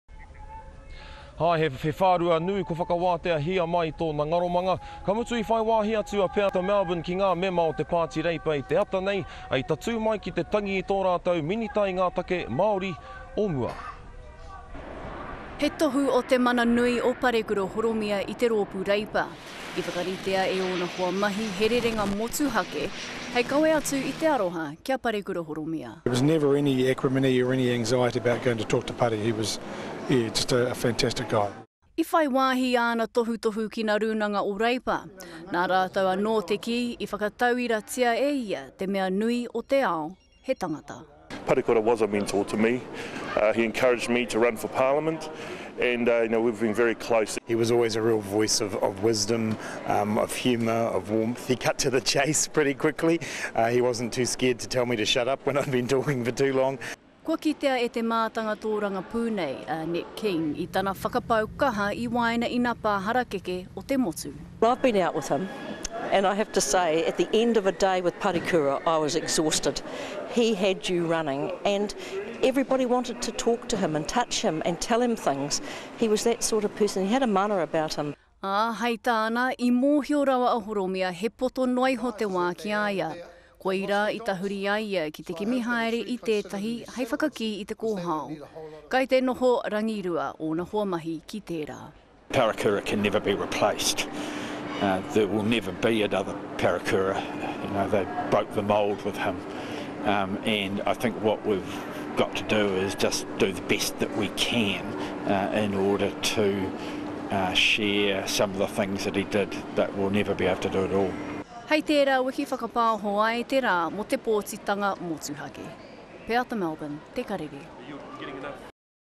Labour Party MPs share memories of an 'irreplaceable' man
The Labour Party also arrived today to grieve their fallen colleague. He leaves a significant hole for the party to fill that some find it impossible to think of replacing him.